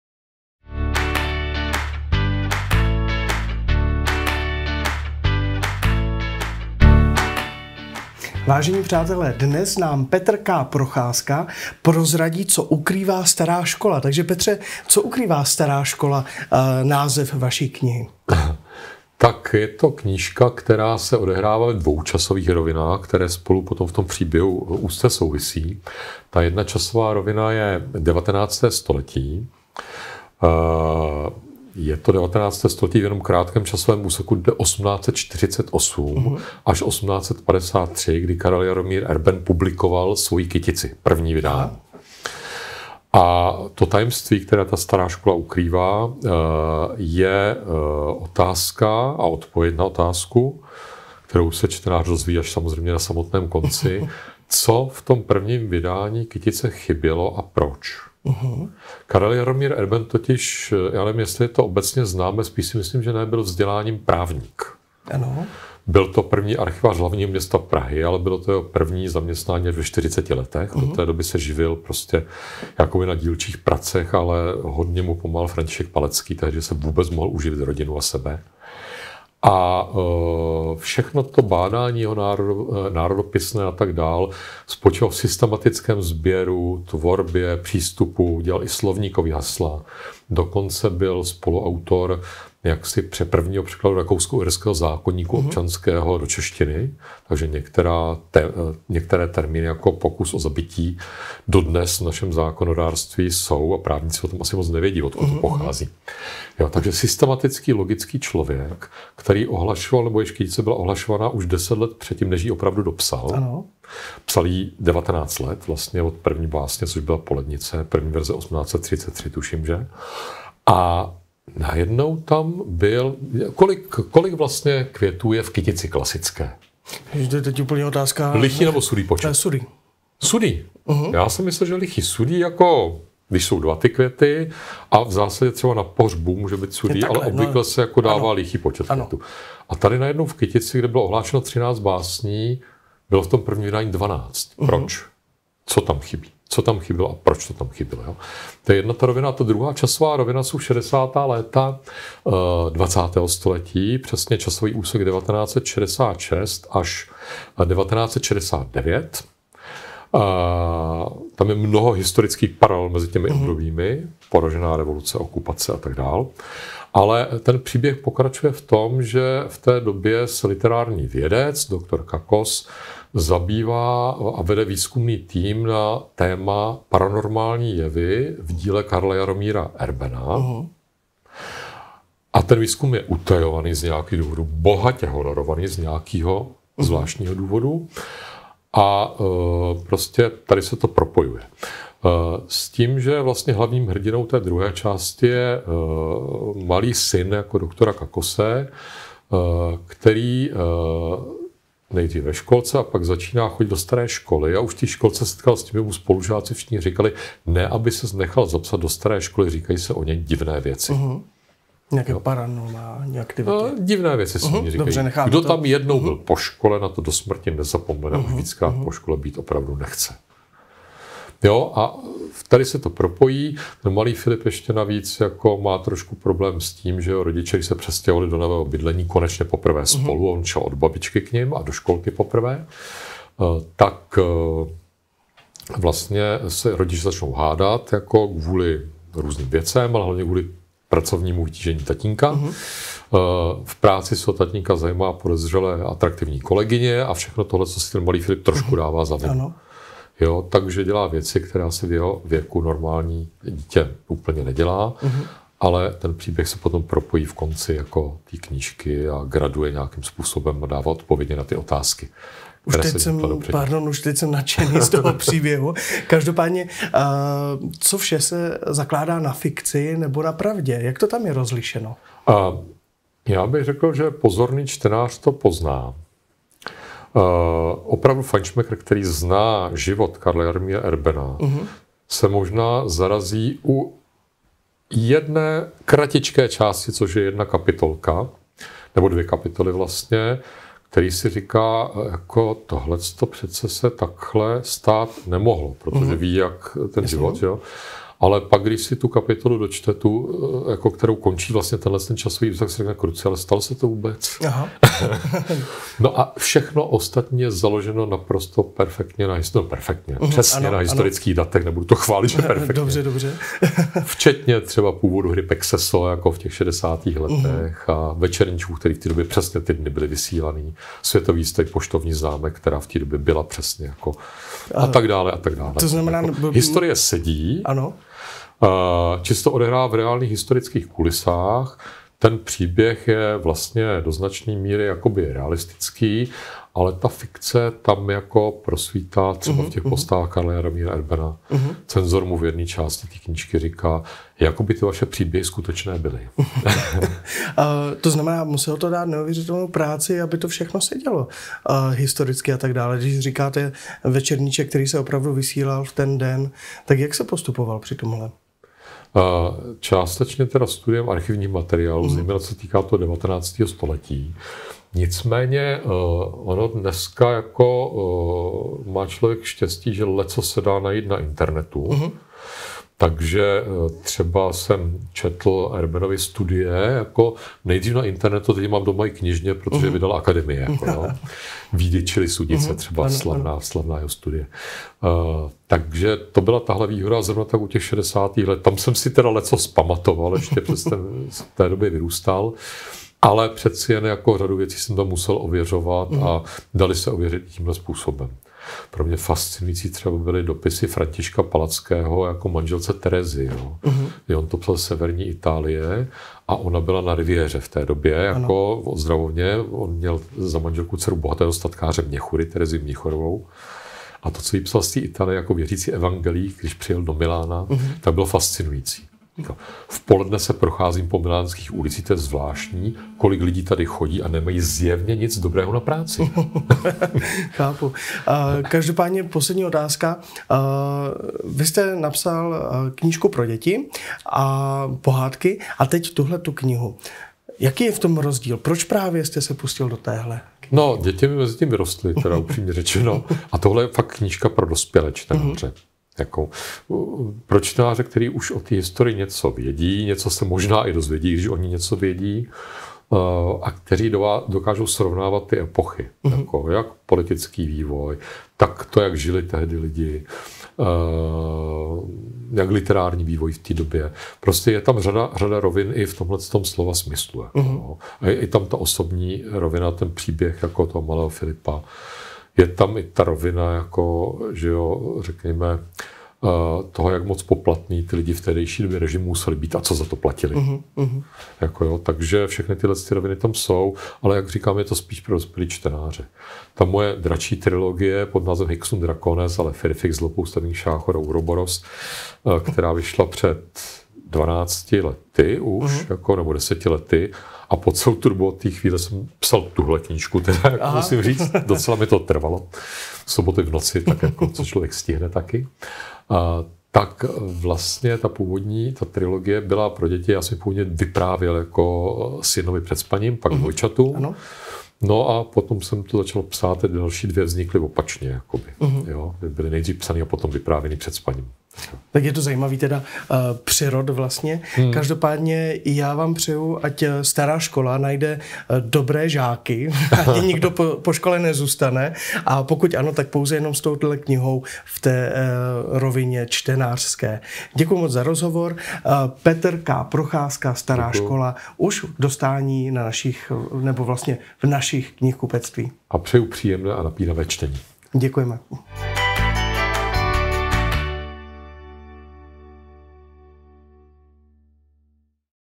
Petr Procházka- Stará škola
Vystudovaný právník a městský archivář u Pražské obce Karel Jaromír Erben byl jako vědec známý přísně systematickým přístupem k národopisnému a historickému bádání i k vlastní tvorbě. Jak se tedy mohlo stát, že po 19 letech od napsání první balady svolil k tomu, aby napjatě očekávané a několikrát odložené první vydání Kytice vyšlo v jiné než zamýšlené podobě? Děj fantaskního románu, který se odehrává na přelomu 40. a 50. let 19. století, a ve druhé polovině 60. let století minulého, se točí kolem jediné otázky. Která z původně zvažovaných balad v prvním vydání chybí a proč? Zatímco ve vědecké práci doktora Kakose, jenž se zabývá ohlasem paranormálních jevů v Erbenově díle, se vrší otazníky, jeho syn Filip má přízemnější starosti. Přepracovaný tatínek je věčně v práci, kde se o něj podezřele zajímá atraktivní kolegyně. Rodiče se po přestěhování do nového bytu hádají a Filipa místo k babičce, na kterou je zvyklý, teď najednou strčí do školky! Proč jen ho kamarádi varují, aby se do první třídy za žádnou cenu nenechal zapsat do Staré školy? Co strašného se skrývá v hlubinách jejího sklepení, před kterým se třesou poškoláci? Zachrání Filip rozpadající se manželství rodičů a podaří se mu té záhadě přijít na kloub? A jaká bude cena, kterou za to zaplatí?